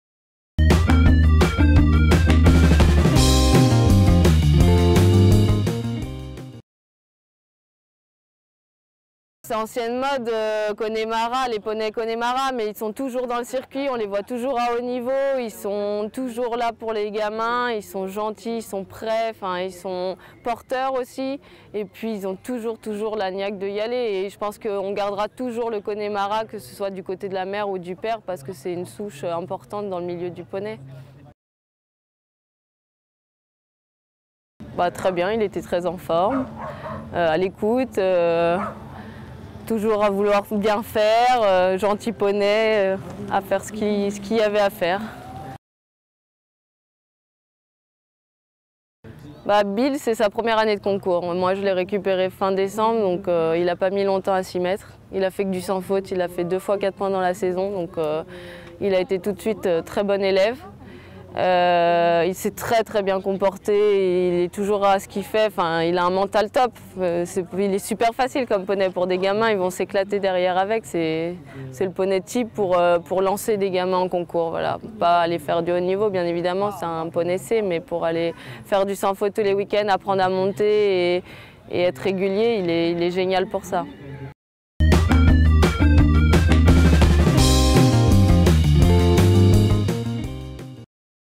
Sologn'PONY 2016 : Bhil du Villon, champion des 5 ans C
Audrey Batillat revient pour nous sur la victoire de Bhil du Villon lors du Sologn'Pony 2016. Ce poney est à vendre sur SHF Market.